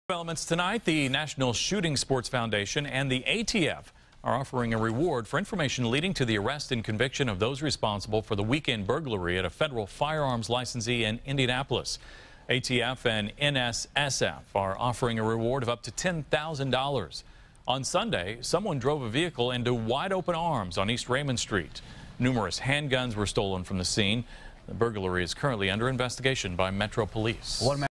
$10K reward offered in Indy gun store theft
Two national groups offered a reward Tuesday to help solve the latest gun theft in the Circle City.